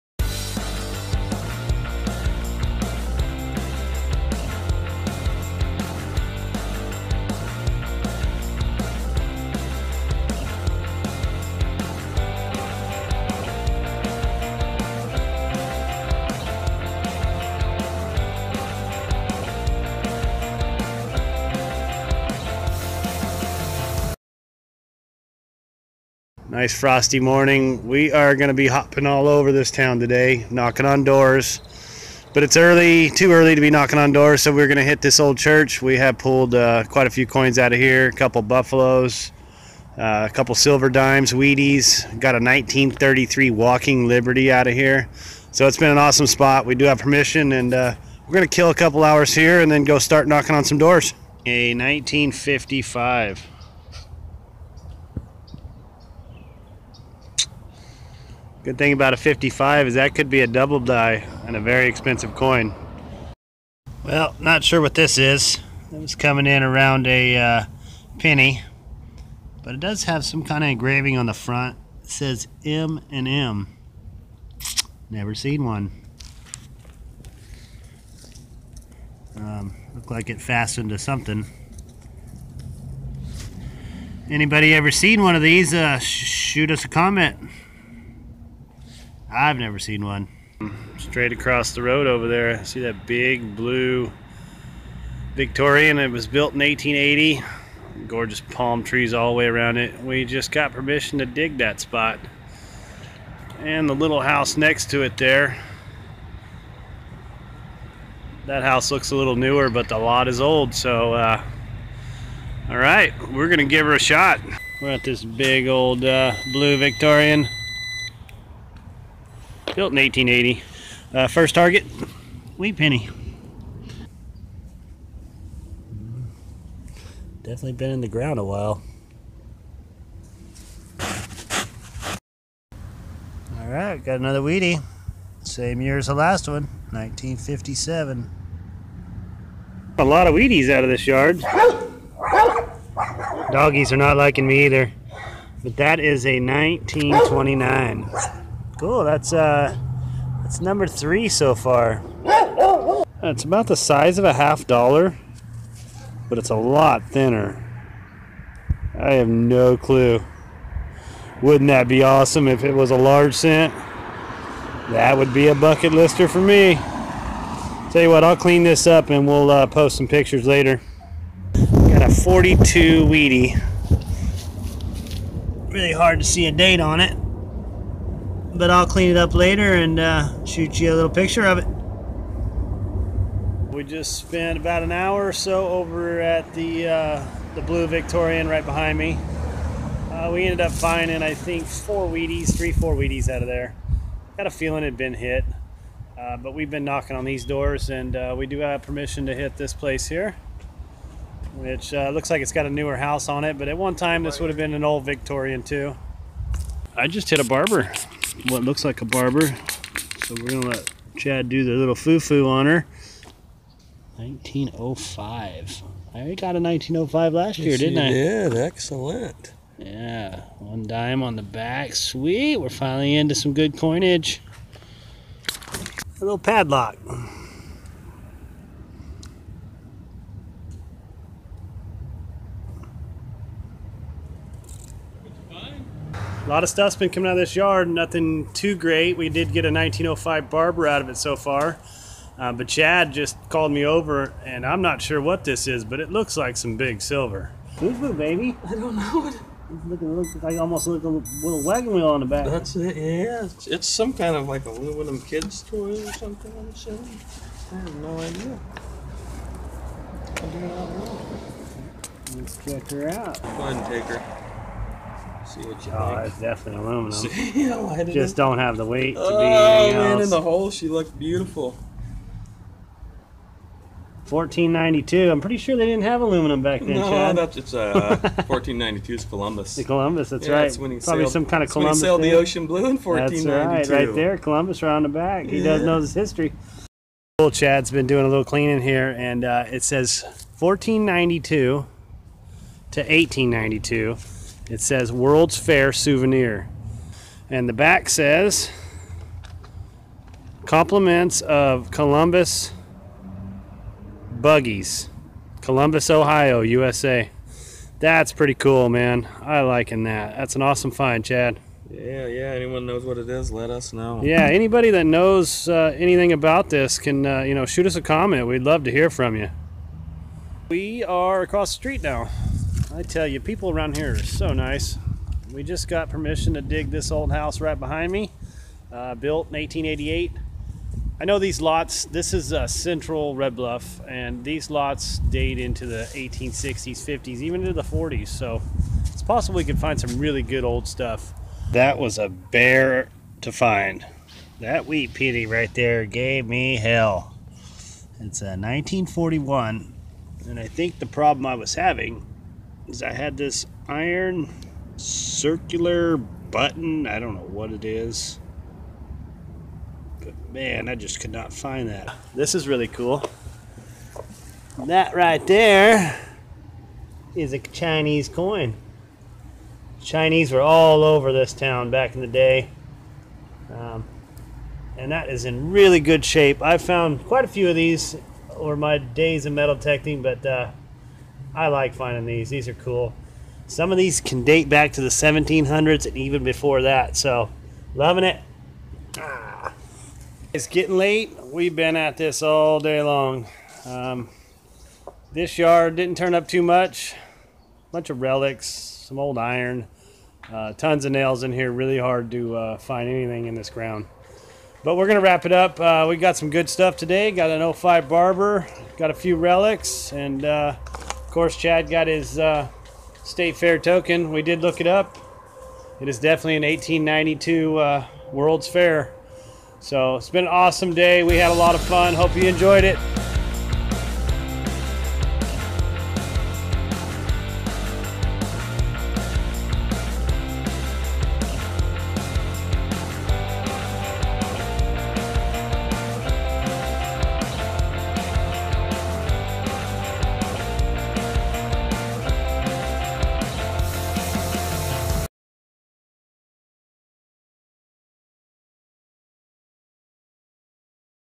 Metal detecting 1880,s Home..World Fair Token!!
Dirty Dan and Mad Chad metal detect some 1880's homes in Red Bluff California, and make some great finds!